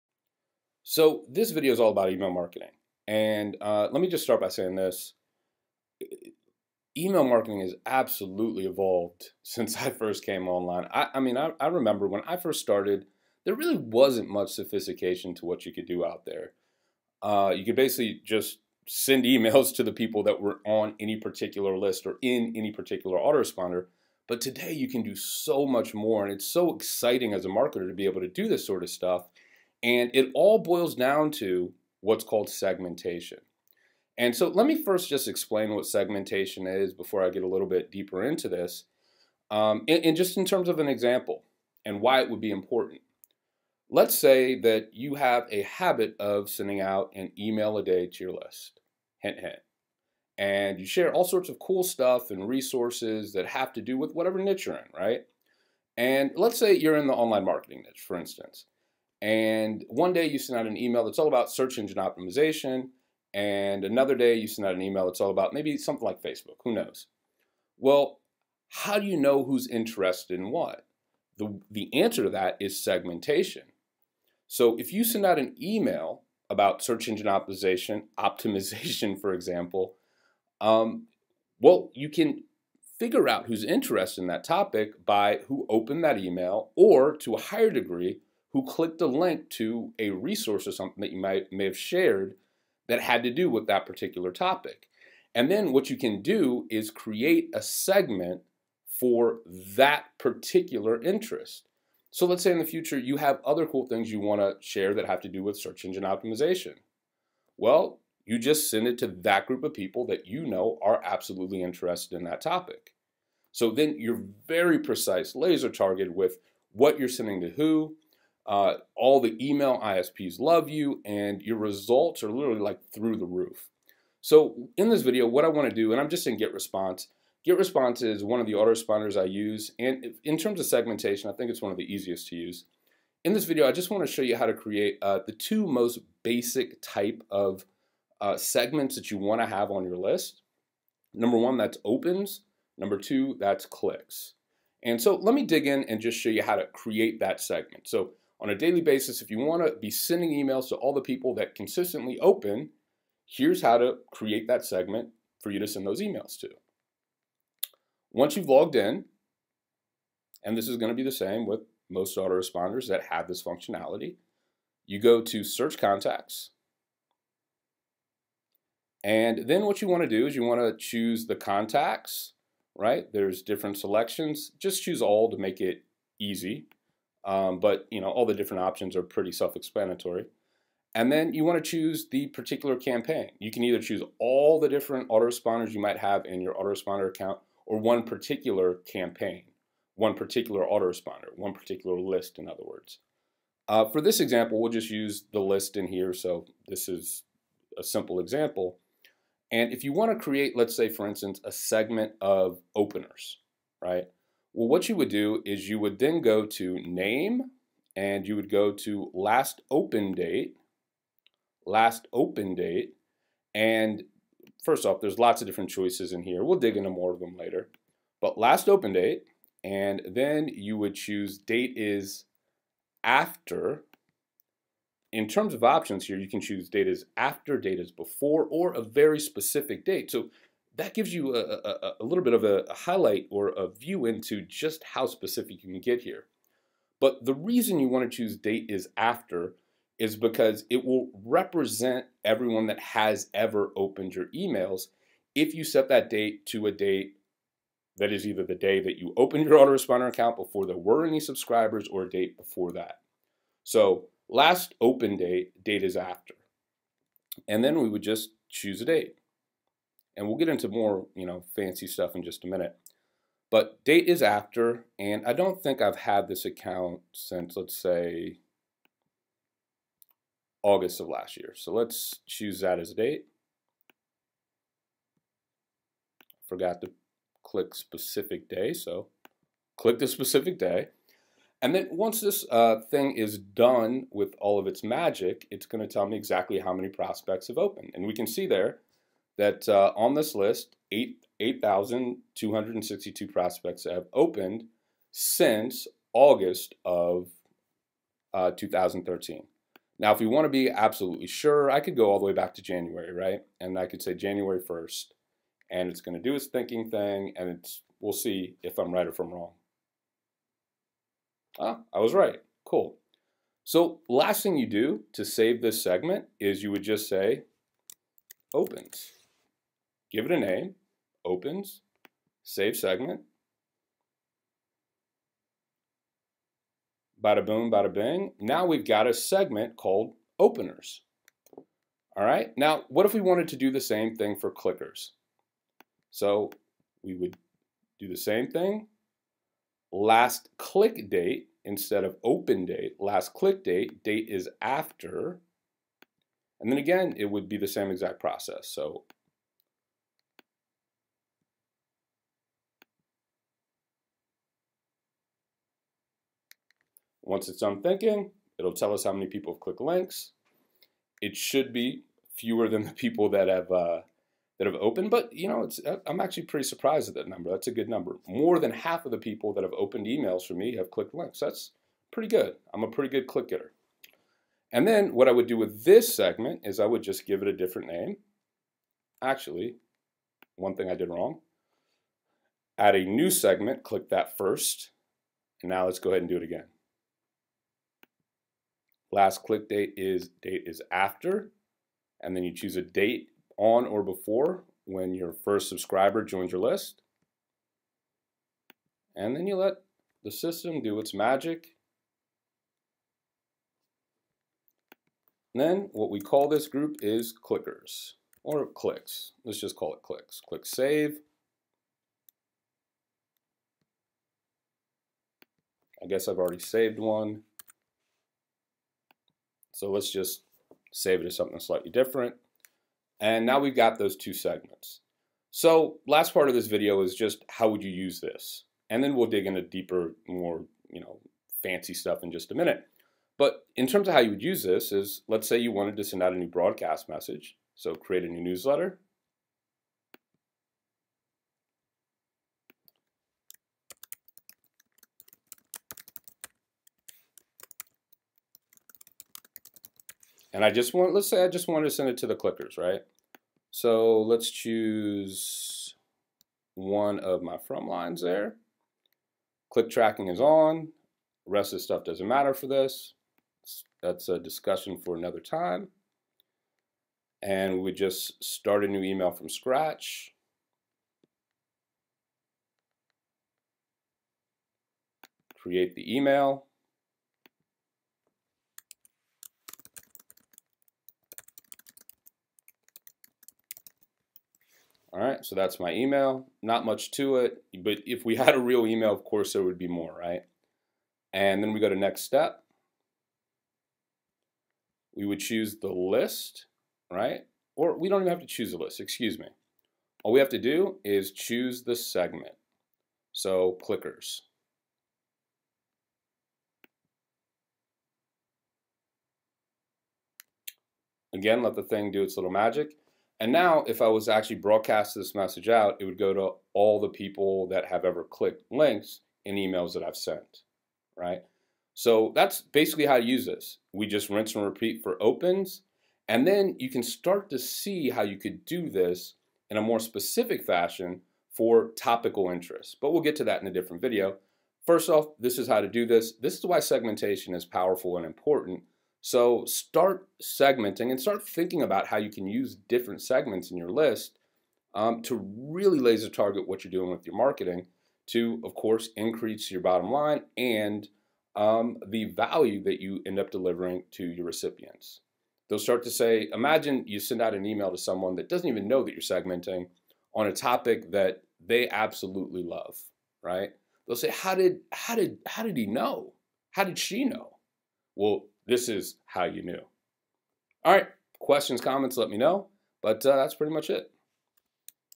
Getresponse - The Why & How Of List Segmentation
- Here's the basic why and how of list segmentation for email marketing using GetResponse as an autoresponder.